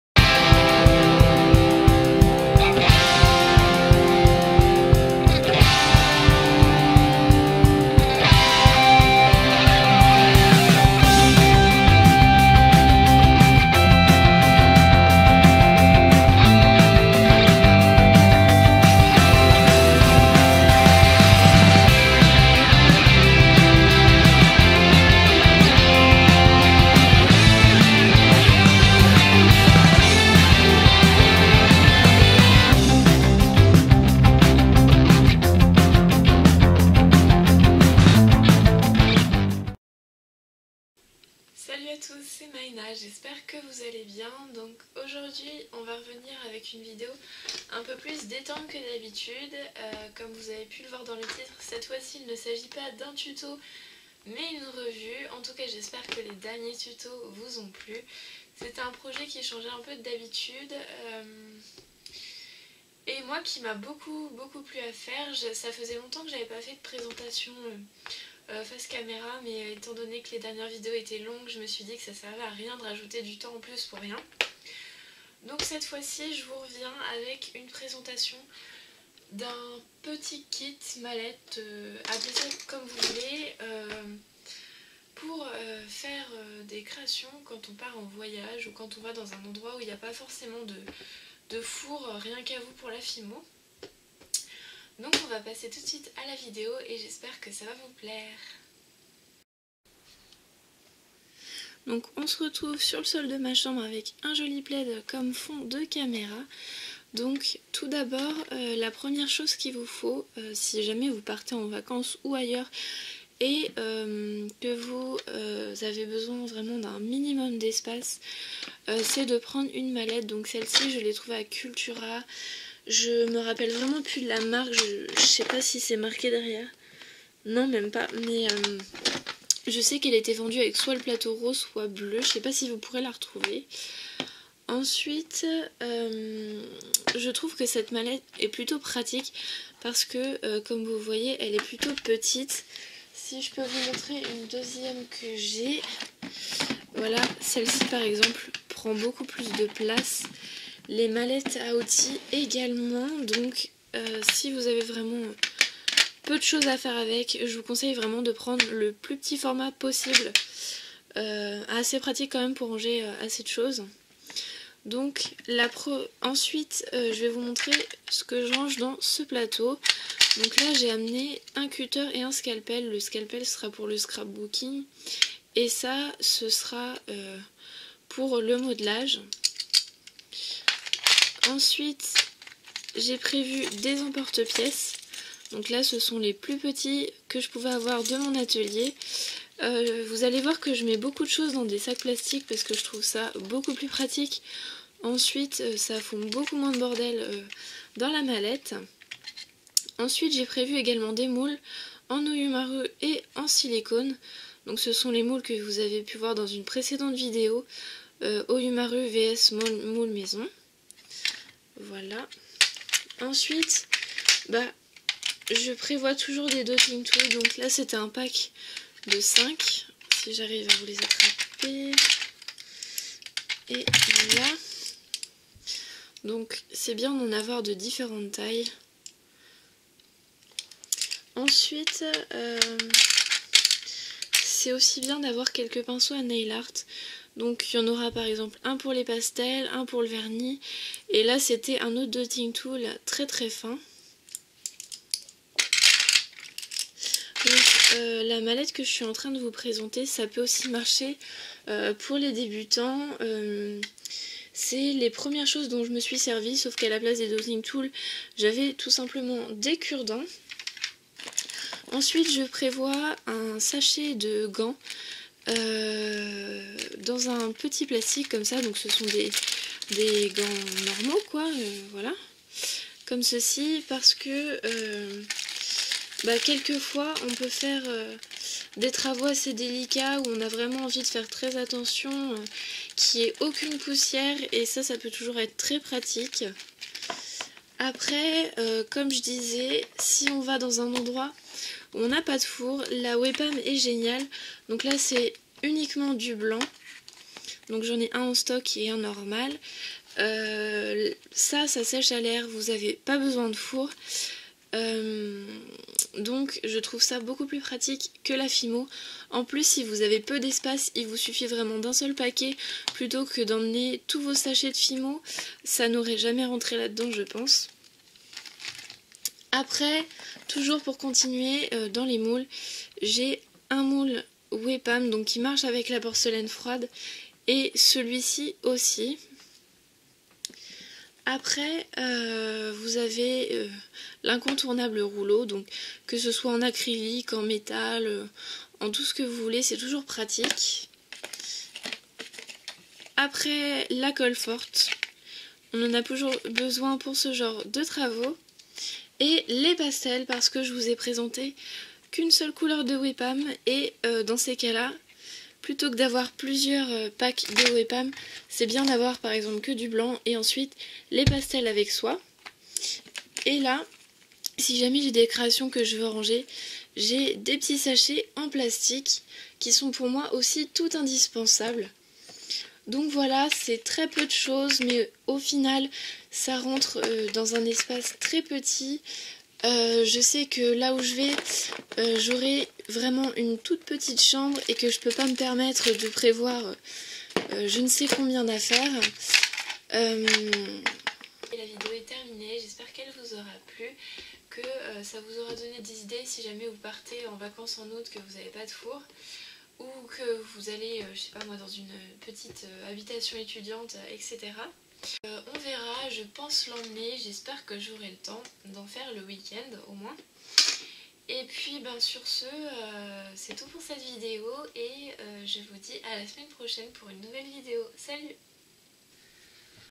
[Revue n°5]: Kit de survie créatif !
Dans cette vidéo, je vous présente mes indispensables, le minimum de matériel à emporter en vacances lorsque vous voulez créer, mais que vous n'avez ni beaucoup de place, ni four ! Enjoy ;)

Maïna's Créatime  2016 ©